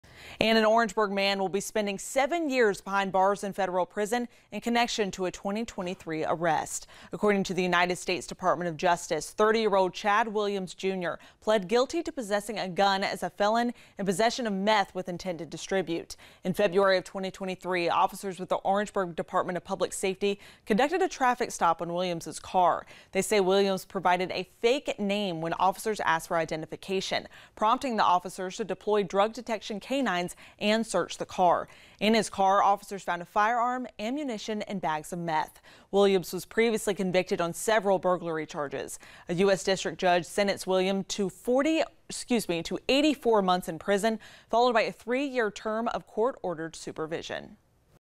VIDEO: Orangeburg man sentenced on federal gun, drug charges
An Orangeburg man will spend the next seven years behind bars after a traffic stop resulted in officers finding guns and drugs intended for distribution.